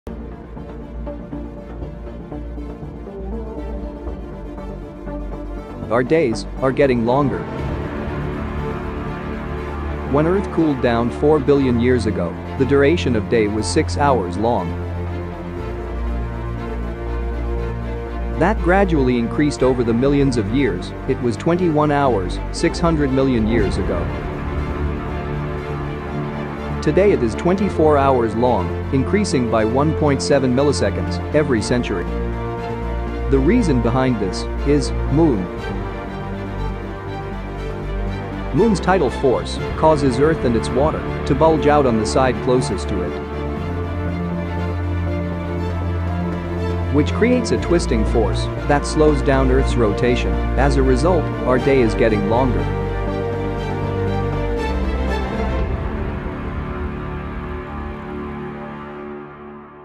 Earth days are getting longer !😲 [ Stellar Talks | Season Finale ]
Earth DAYS are getting longer slowly. It was just only 6 hrs day light when earth was newly formed 4 billion years ago. So what's happening with the earth rotation that is making day longer, find out the reason in this Video (short).
This is the last video of the season 1. If this series gets enough love & support i'll bring season 2 & with much greater quality